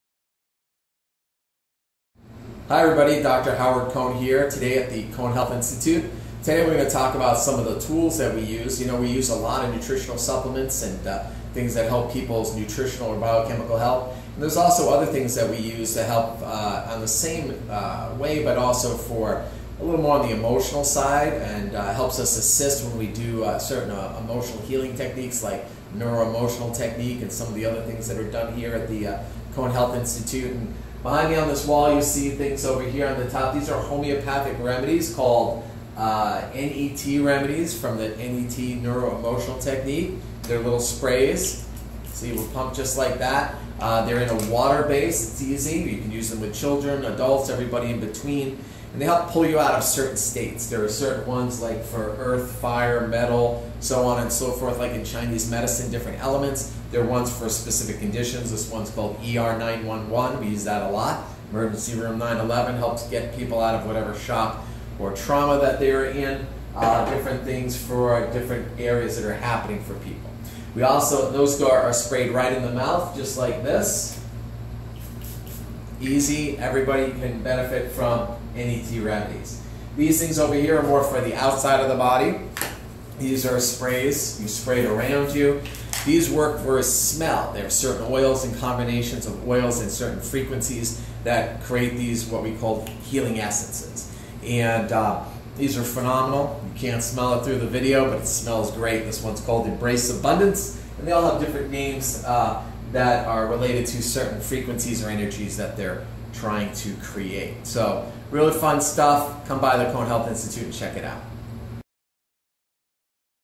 Homeopathetic Sprays Can Help You with Dr. Howard Cohn, DC
Discover how homeopathetic sprays can help you with Dr Howard Cohn, DC at the Cohn Health Institute. Learn about NAET, allergy elimination technique and how our outer and inner body sprays are used to relieve your allergies.